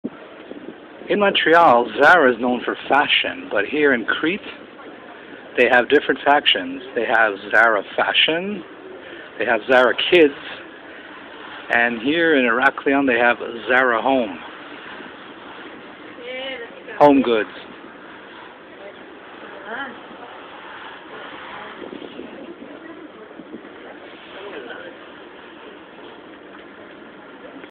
EMMANUELS MONTREAL GREECE - ZARA STORE in GREECE
The ZARA Store in Greece is distinguished in 3 types of specialty stores - 1 that spotlights Fashion - (ZARA) - 1 that spotlights Kids fashions and kids merchandise ( ZARA KIDS) and 1 that focuses on household items ( ZARA HOME )